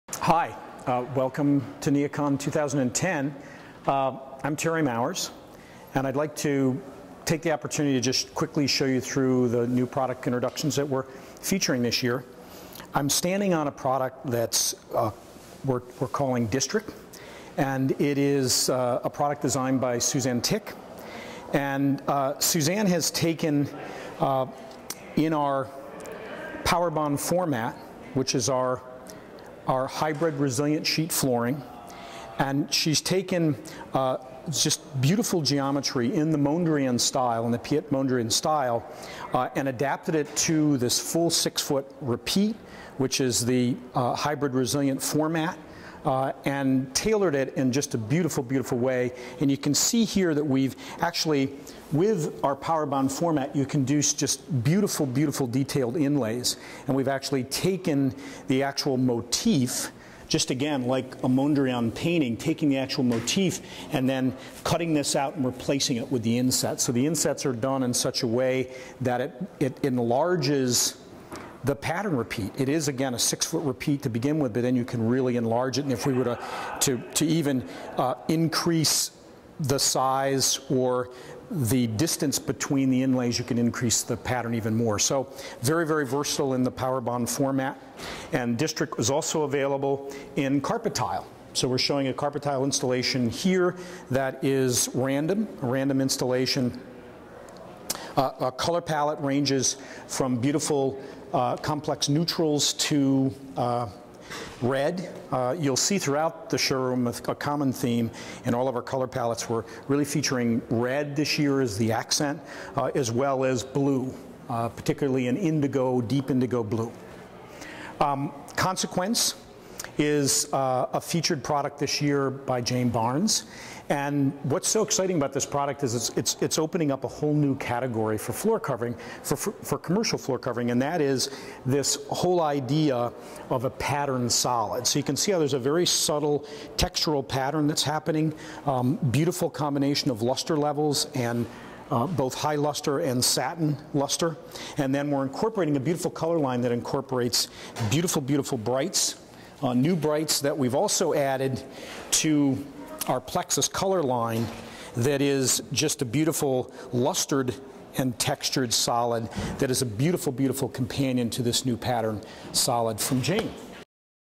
Tandus Flooring at NeoCon
Terry Mowers, Creative Director at Tandus Flooring, welcomes you to the showroom and displays some of Tandus Flooring's latest and greatest products including Powerbond--the industry's first hybrid resilient sheet flooring.